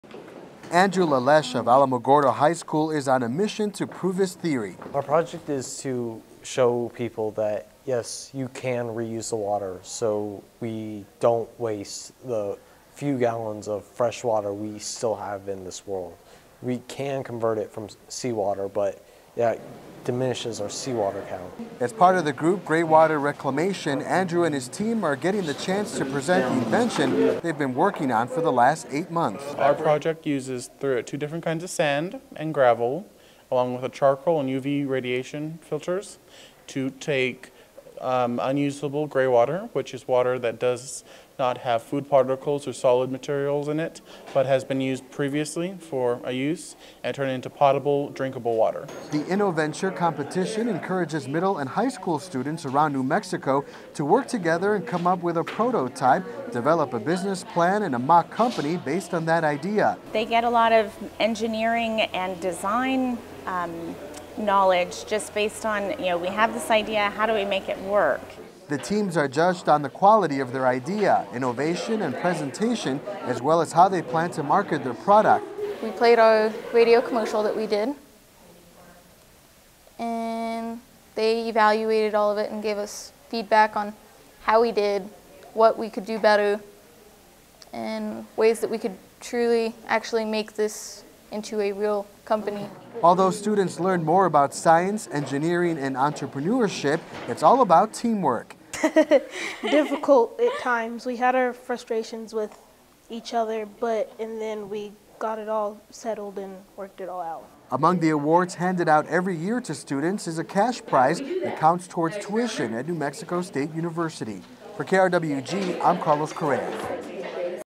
Student Inventions
03.08.11 (LAS CRUCES) -- Students from around the state are working on creating the next big thing.  As KRWG's Carlos Correa reports -- these young inventors are also learning what it takes to start their own business.